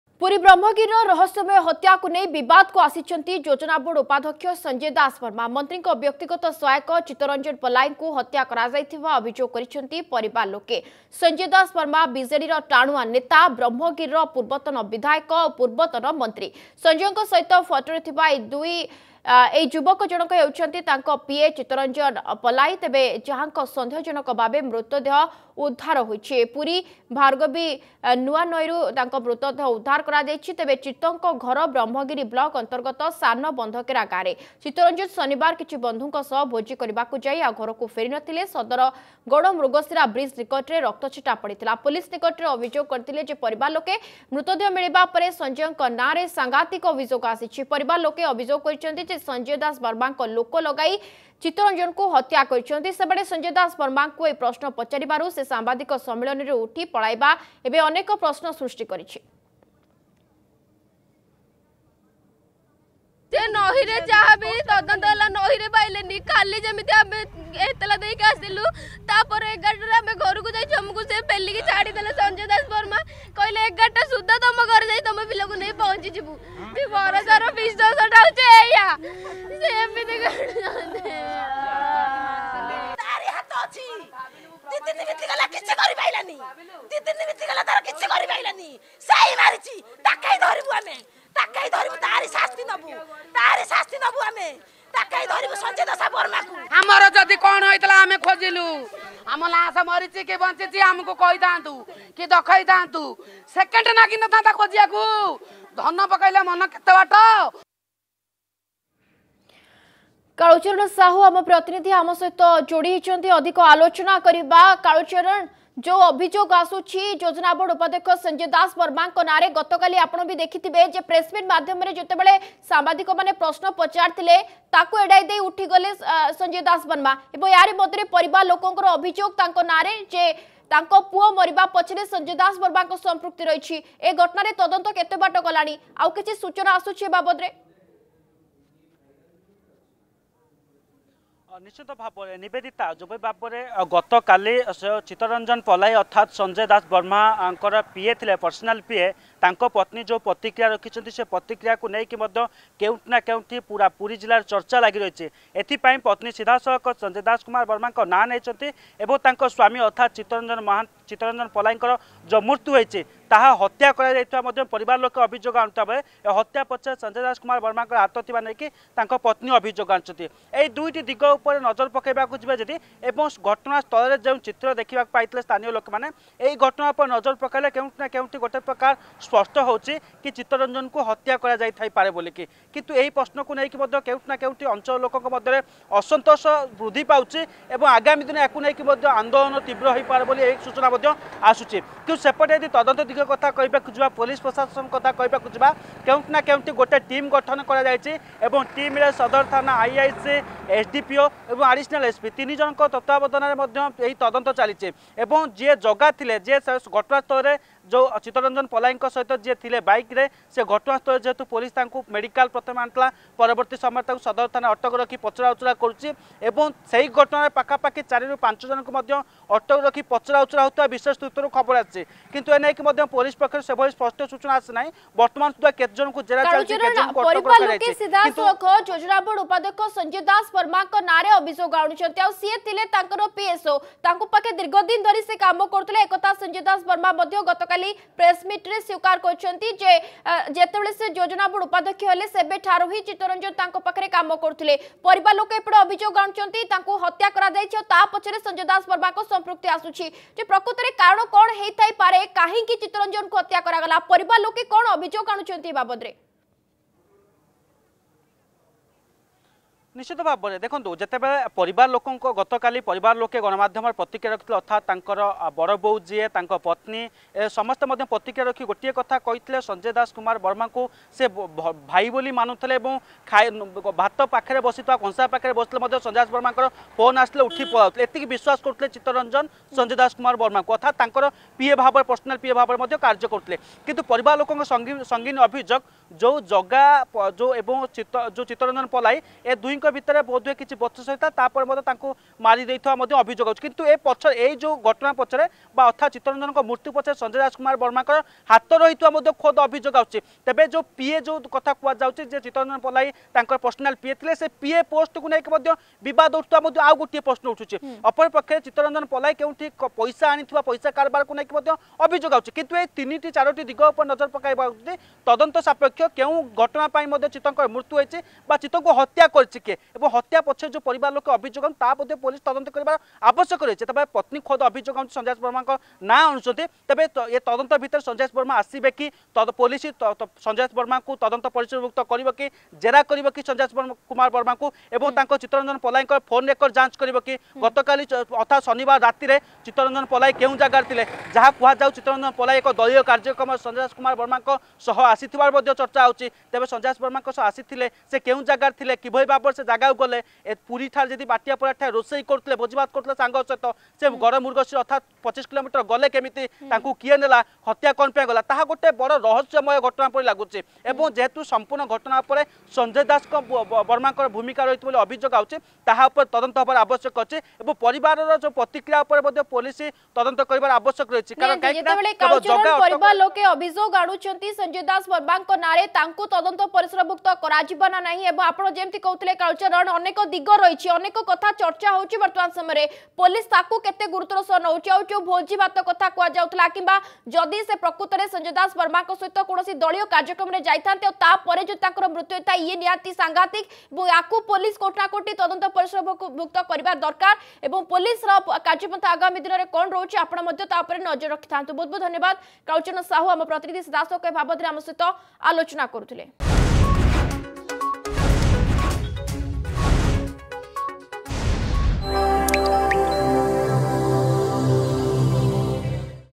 Brahmagiri Murder Case: Sanjay Das Burma in Controversy Over Mysterious murder of his PA
Sanjay Dasbarma, deputy chairman of the planning board, is embroiled in controversy over the mysterious murder of his PA Chittaranjan Palai.  Family members have accused Sanjay Das Burma for the Murder of Chittaranjan palai.